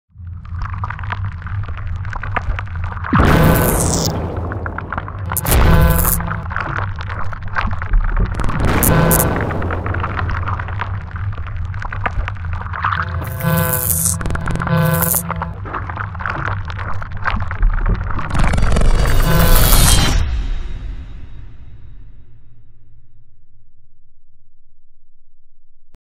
Electric Brain Animation
Testing out some new elements and effects.  Tis' a work in progress.  If you watch real closely, the brain is moving.